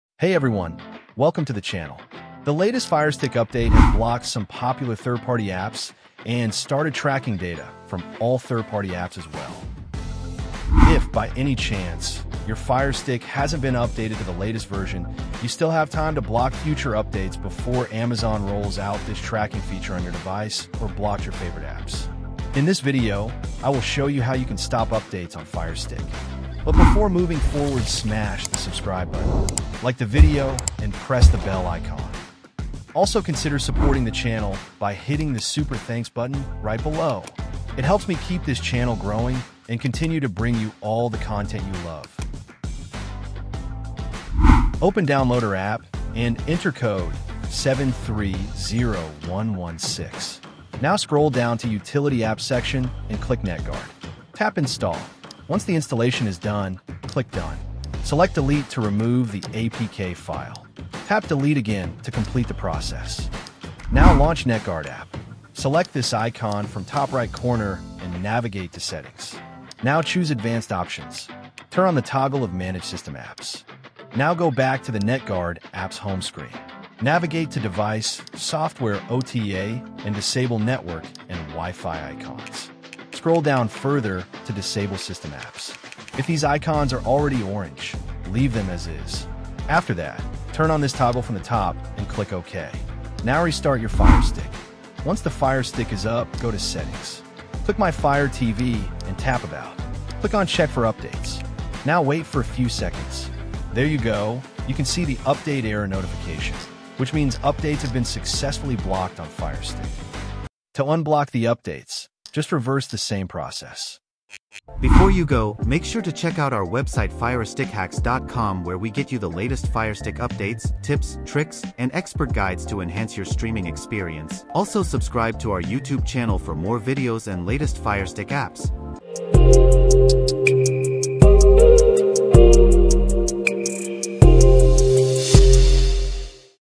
Stop Amazon's New Firestick Update From SPYING on You! (Disable Auto Updates Now)
In this video, I'll show you how you can stop amazon automatically updating your firesticks to the latest version and also stop amazon new data collection feature.

✅The Only Best Downloader Code : 730116